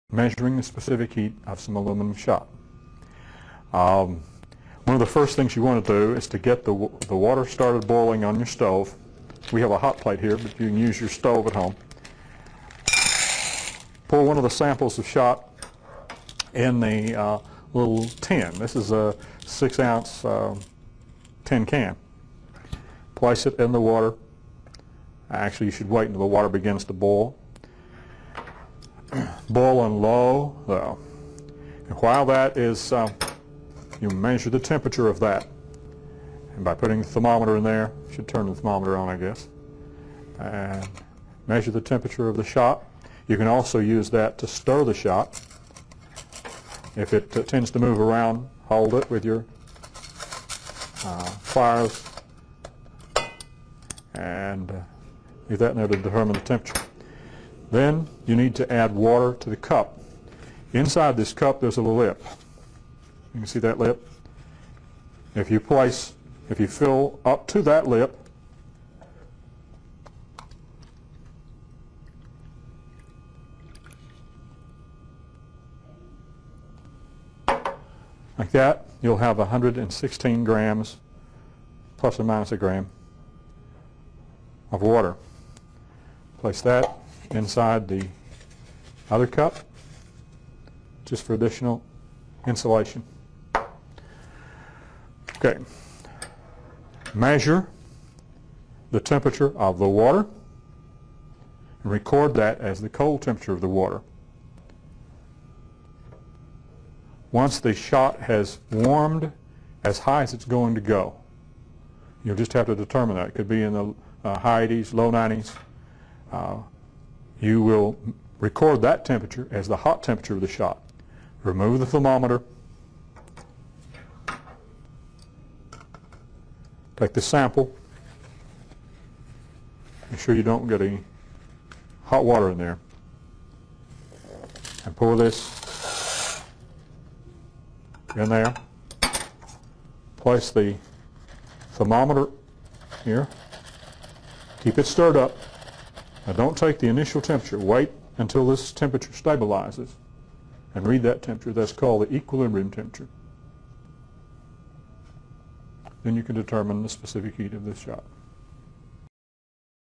Specific Heat
This video models the data collection used in the Specific Heat Lab for online PHYS1030.